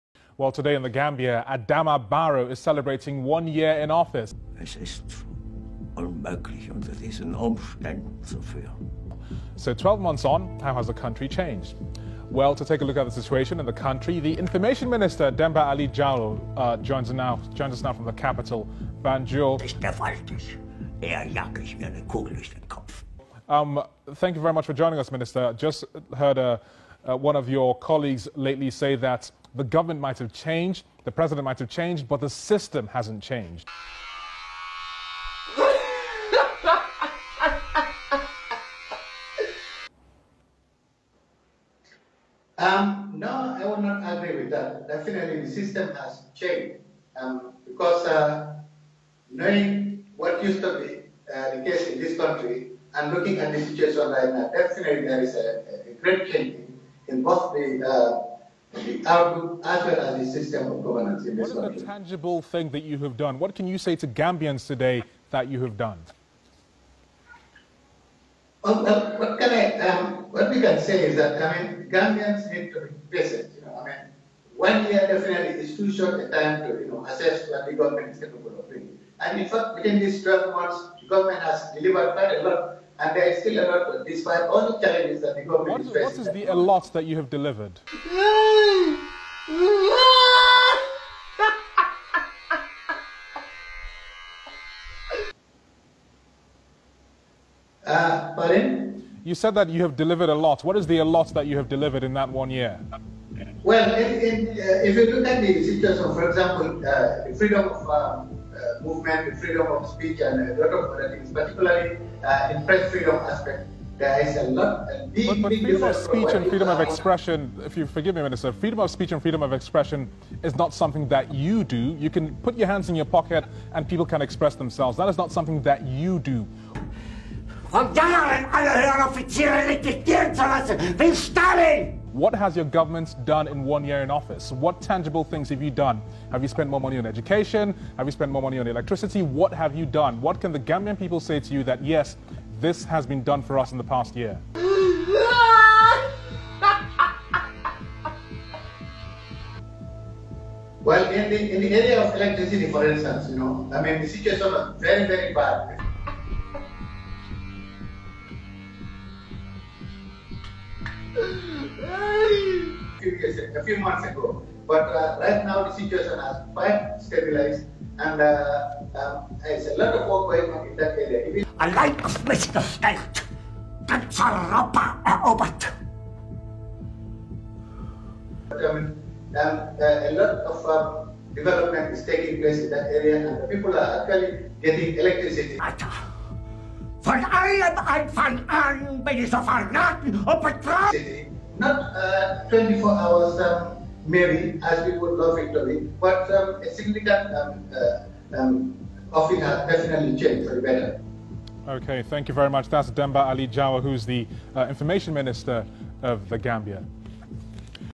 Sulayman Shyngle Nyassi posted a video to his Sulayman Shyngle Nyassivia torchbrowser com
A 'CAR-CRASHED INTERVIEW' WITH THE INFORMATION MINISTER (D A JAWO) OF THE FAILED AND CRUMBLING REGIME IN BANJUL...

FOR 12 GOOD MONTHS NOW - THEY'VE DONE NOTHING AND I MEAN NOTHING TO SHOW IT FOR BUT STEALING, LYING AND BUYING TIME WITH THE GAMBIANS AT THIS HOUR. THEY HAVE WOEFULLY FAILED THE NATION SO THEY